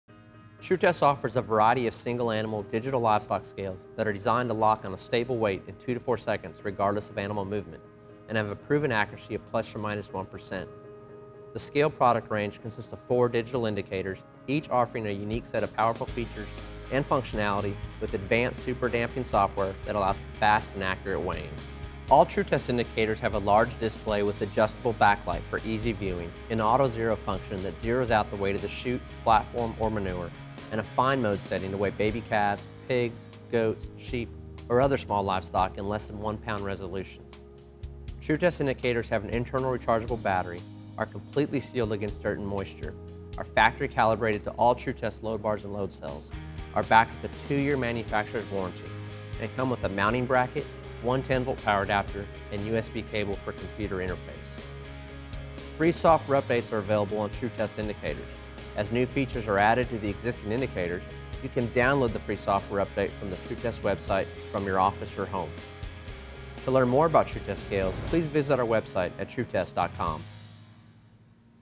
Tru-Test Indicators Overview
Tru-Test indicators are the fastest, accurate, most versatile and durable indicators in the world. All our indicators feature Superdamp™ technology, unique to Tru-Test, to record accurate weights quickly, even with the liveliest animals. Within 3-6 seconds our weigh scales lock on a weight, and a bright stable light lets you know when it's ready to be recorded. An auto zero function also ensures any accumulation of animal dirt or manure is zeroed after the animal leaves the scale. Tru-Test indicators are compatible with all Tru-Test load bars.

All Tru-Test indicators have:

Internal rechargeable battery
Mounting brackets
2-year warranty.

And all Tru-Test indicators feature:

IP67 environmental rating
100% water and dust proof